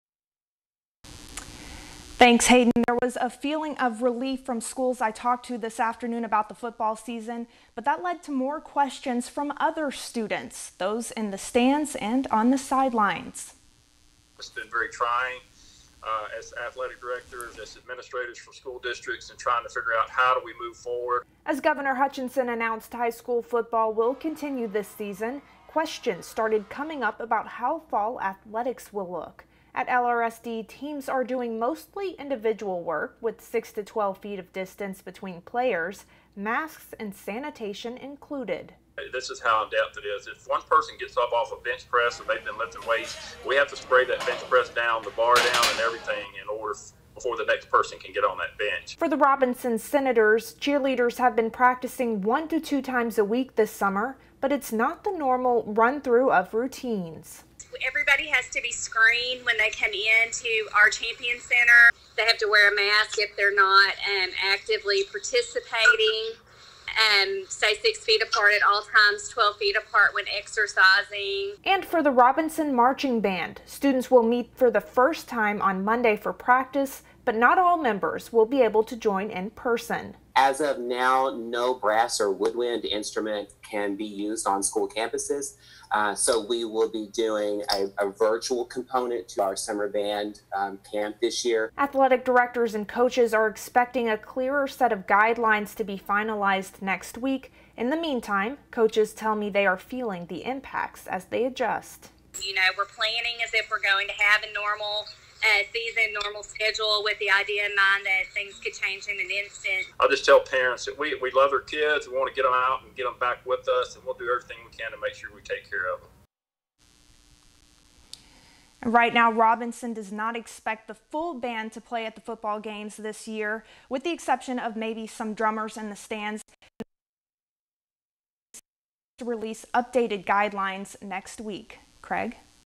What will Arkansas high school cheer teams and marching bands look like this fall?
Governor Hutchinson's decision doesn't just affect football. It also impacts other fall athletics, like cheerleading, marching band, volleyball, and more.